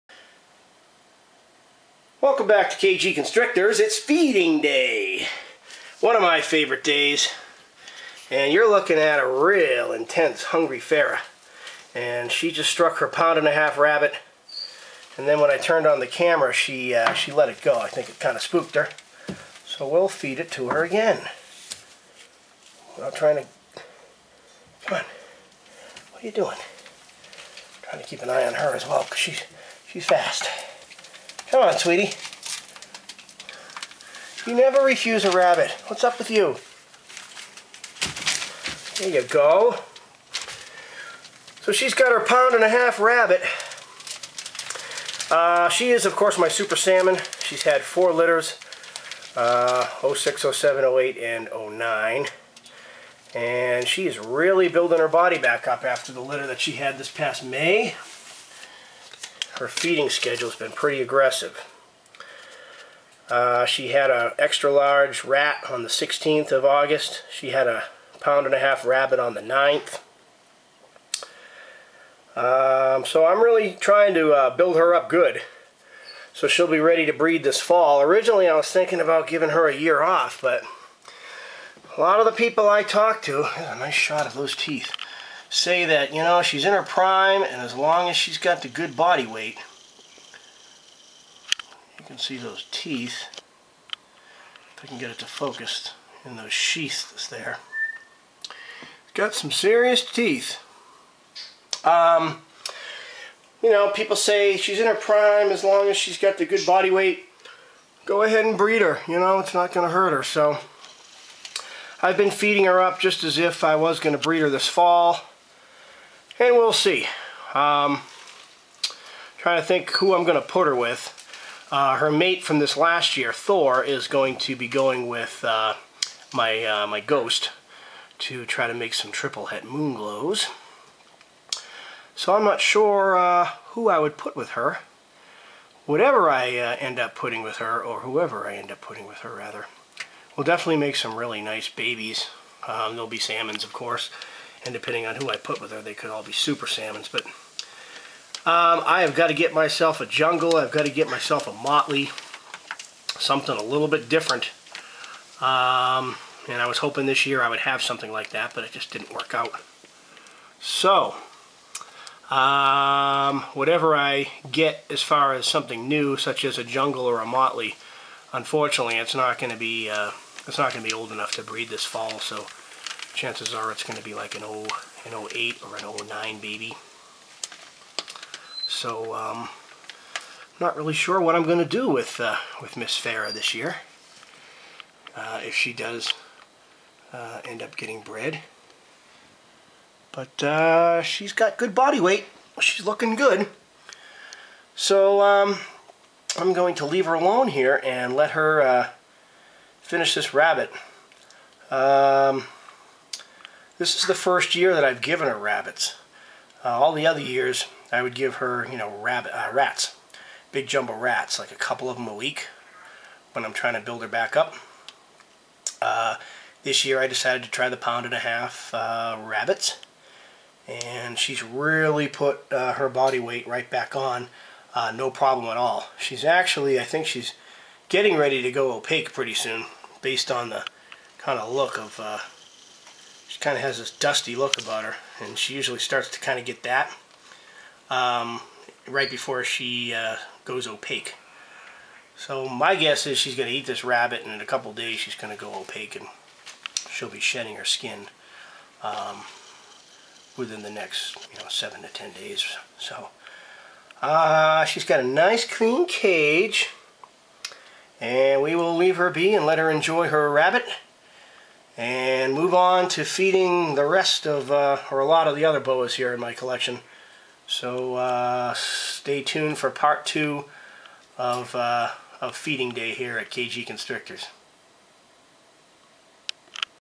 Farrah Loves Her Bunnies
My 2003 Super Salmon Female enjoys
a pound and a half rabbit.